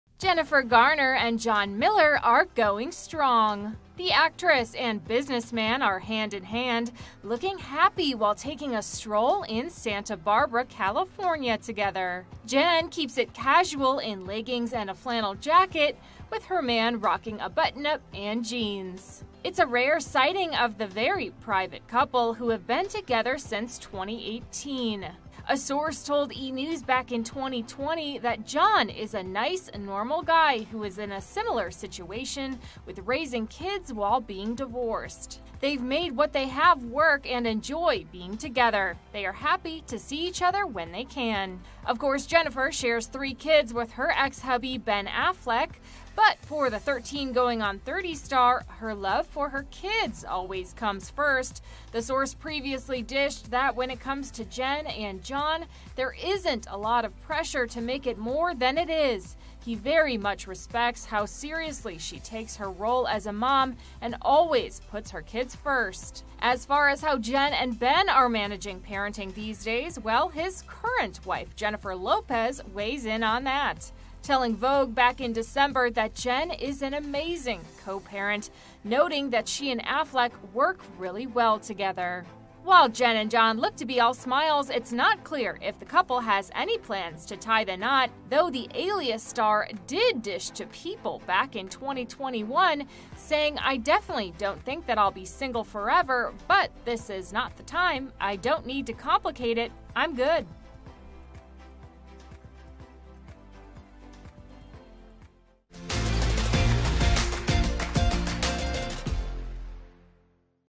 Jennifer Garner and John Miller's PDA-Filled Outing | E! News
Jennifer Garner and boyfriend John Miller are seen walking hand-in-hand during an outing in Santa Barbara, California. Watch!